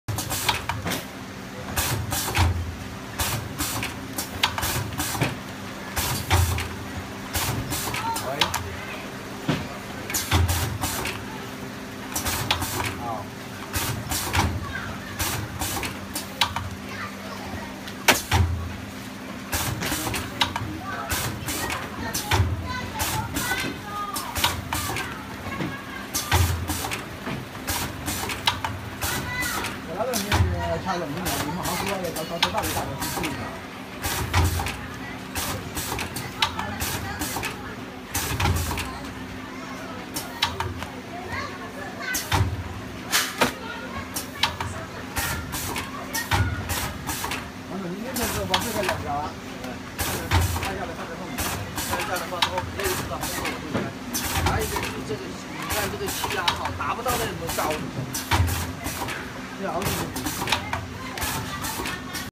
INNOVO A Four heads paper bag dotter
It can dotter paper bag four bottons at same time
contact person:dora